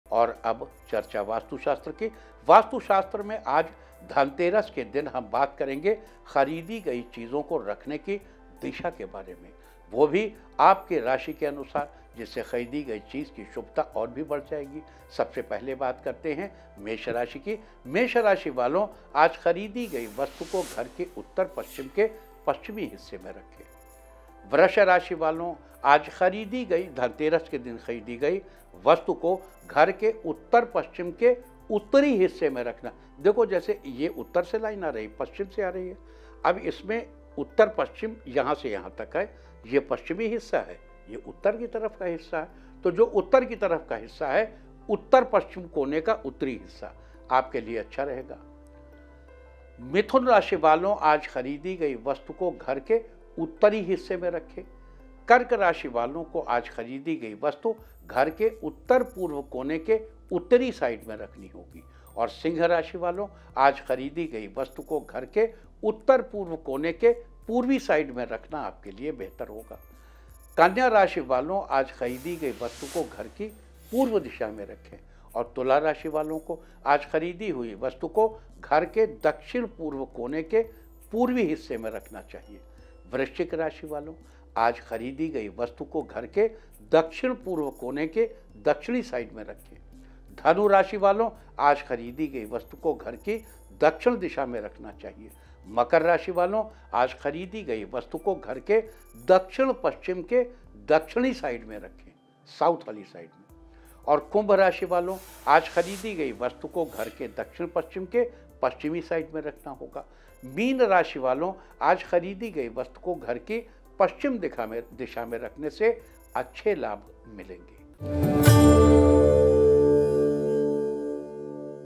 Vastu Tips: Dhanteras पर वास्तु से कैसे संवरेगी आपकी किसमत? जानें Acharya Indu Prakash से
वास्तु शास्त्र घर, प्रासाद, भवन अथवा मन्दिर निर्माण करने का प्राचीन भारतीय विज्ञान है। जीवन में जिन वस्तुओं का हमारे दैनिक जीवन में उपयोग होता है उन वस्तुओं को किस प्रकार से रखा जाए वह भी वास्तु है। वैदिक वास्तुकला के नियमों और निर्देशों का पालन करके, वास्तुकला के कार्यक्रम में आपूर्ति और आयाम की स्थापना की जाती है, जो एक सुखी और समृद्ध आवास का सृजन करते हैं। तो जानिए Acharya Indu Prakash से वास्तु शास्त्र के बारे में India TV Astro पर।